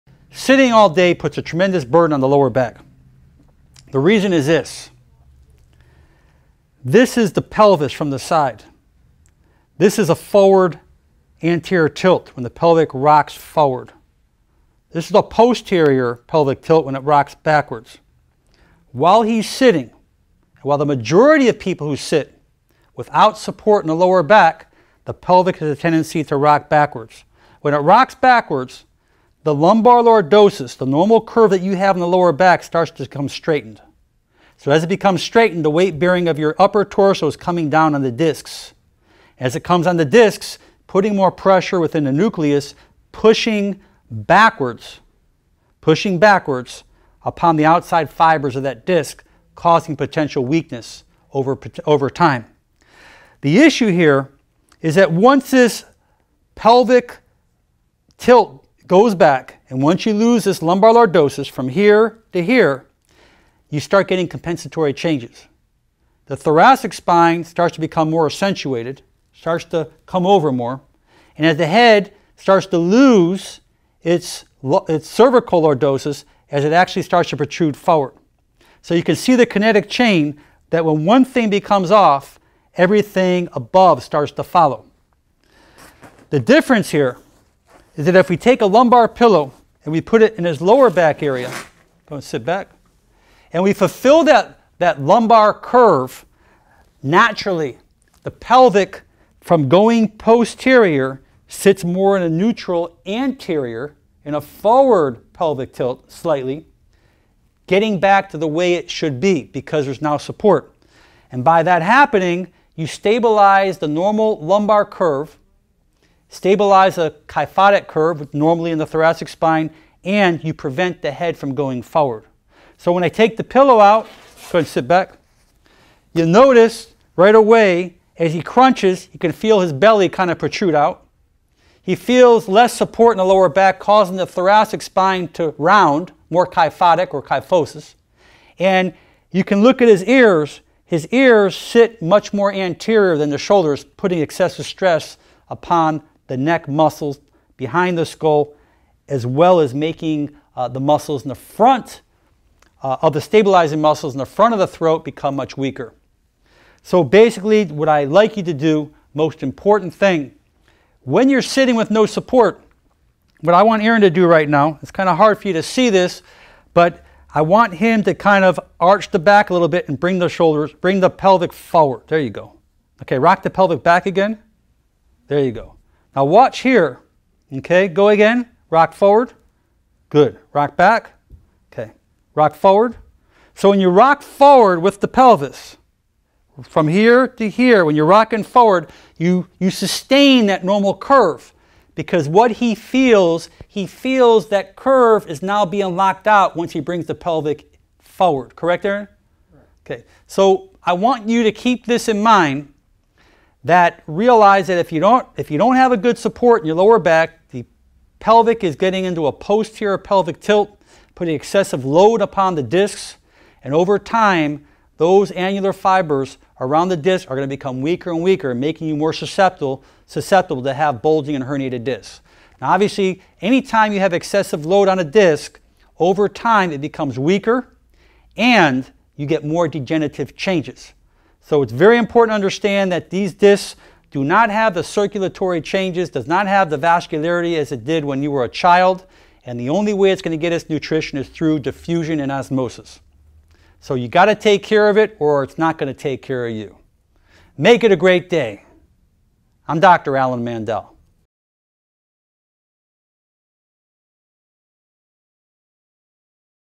How Sitting Causes Posterior Pelvic Tilt Causing Forward Head Posture, Disc Pathology - Dr Mandell
Sitting affects the normal biomechanics of the lower spine (lumbar region) causing the pelvis to move into a posterior tilt.  This affects the kinetic chain up to the neck area causing forward head posture and other disc pathologies.